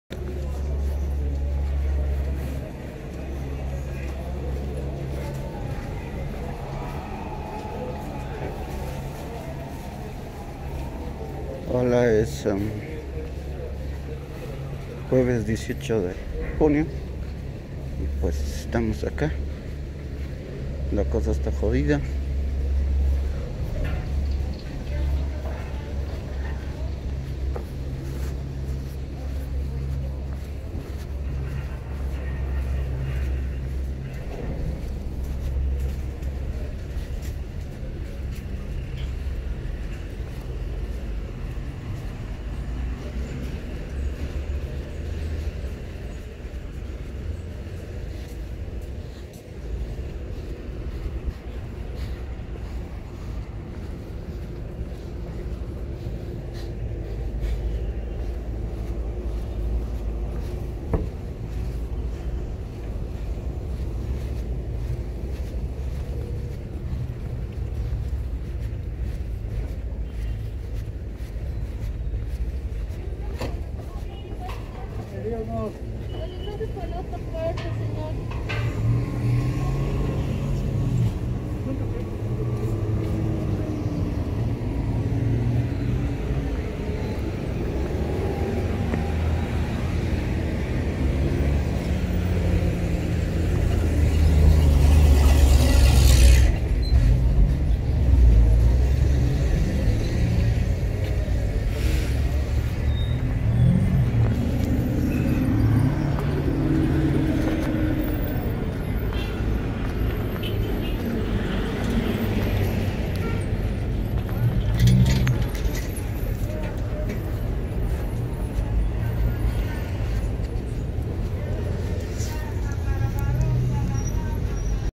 18 de junio de 2020, en la Estación del Teleferico Naranja en Cuarentena Dinámica
Ahora estamos en la Estación del Teleferico Naranja en Plena Cuarentena Dinamica  en la Paz